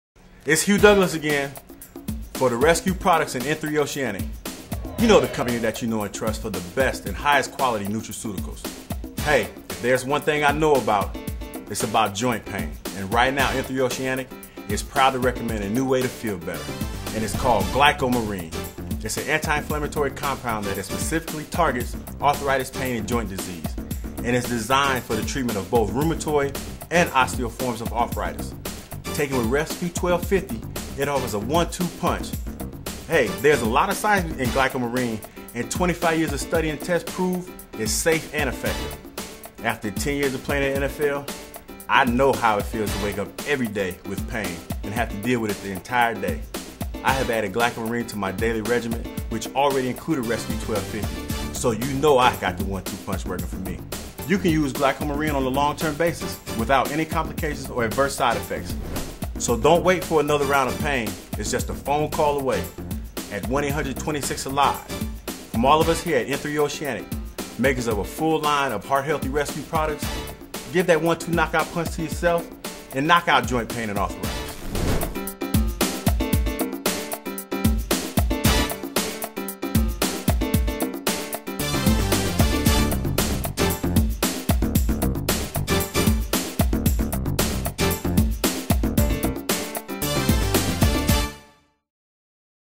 GlycoMarine.wmv
Hugh Douglas sounds off about GlycoMarine available through Nature's Shield and Res-Q.